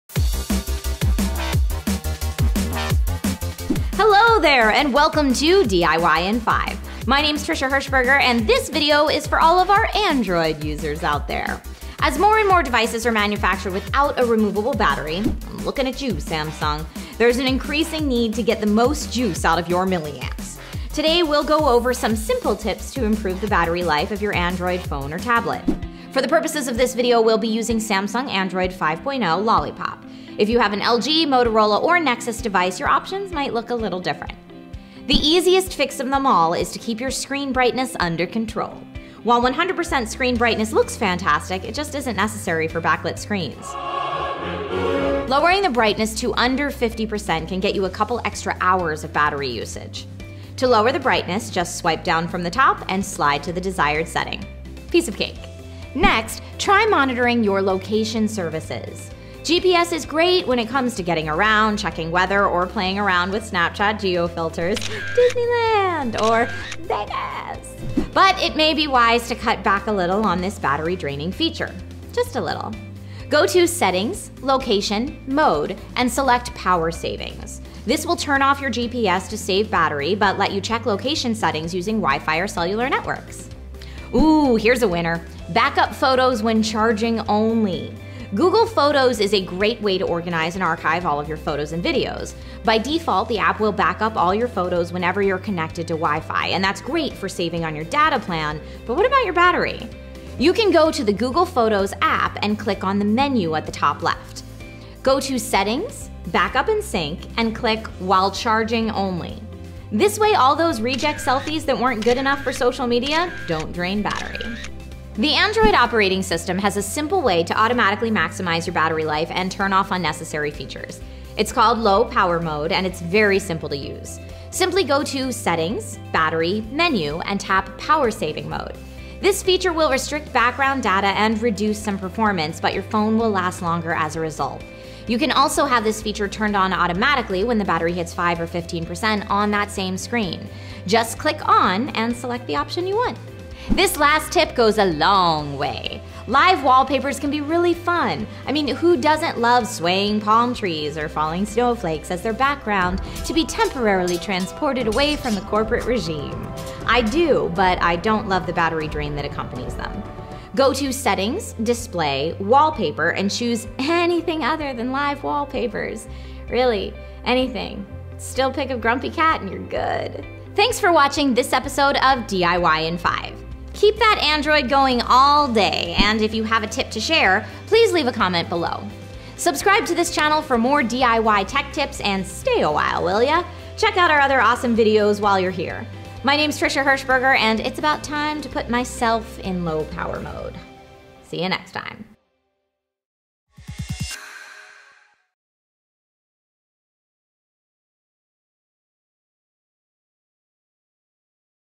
Советы по экономии заряда батареи для устройств на Android | Kingston DIY in 5, эп. 6
Устройства на платформе Android позволяют вам общаться в любое время и в любом месте, но могут ли они проработать без подзарядки целый день? В этом видео мы покажем вам, как продлить работу мобильного устройства под управлением Android 5.0 Lollipop. Приложения, работающие с высокой яркостью подсветки экрана, оказывают большую нагрузку на аккумулятор. Если к тому же включить функцию определения местоположения, то телефон может не дожить и до обеда. Мы покажем вам, как изменить несколько настроек, чтобы обеспечить достаточно энергии для работы на протяжении всего дня.